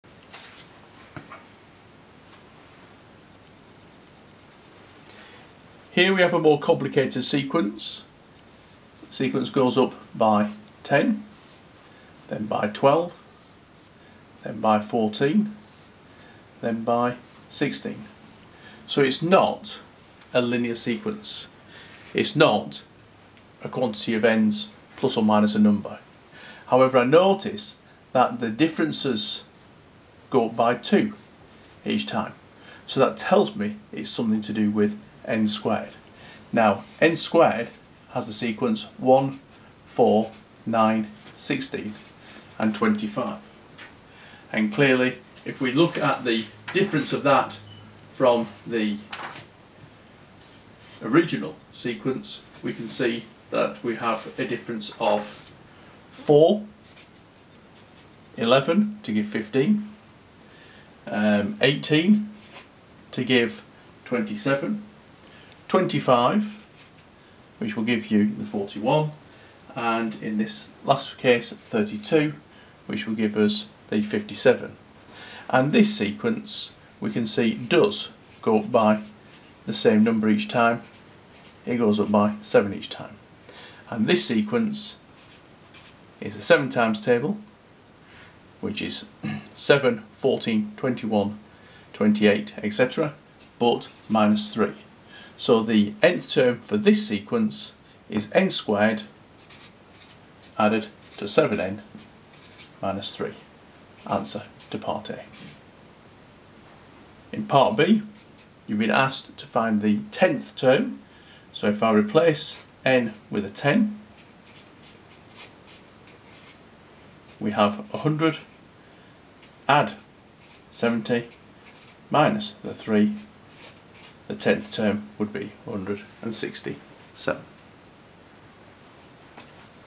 Finding the nth term of a quadratic sequence
Finding the nth term of a sequence which involves the square of n.